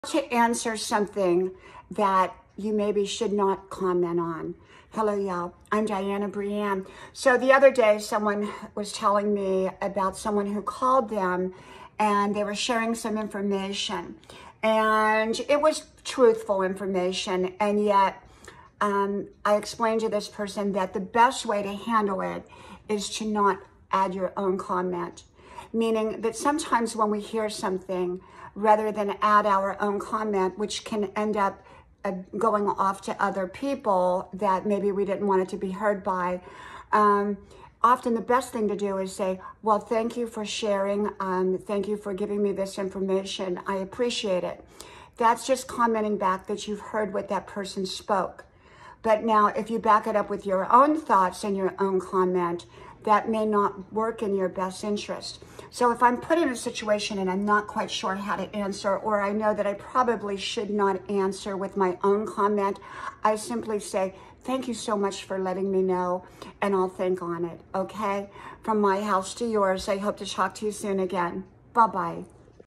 WHAT TO SAY and NOT SAY WHEN YOU DON'T KNOW WHAT TO SAY! How to respond in the best way possible
Motivational inspirational self-empowerment happiness encouragement self-improvement being your best you starting today happiness. When you don't know what to say sometimes the best thing is to say nothing or to say "Thank you for letting me know" and not adding a comment of our own.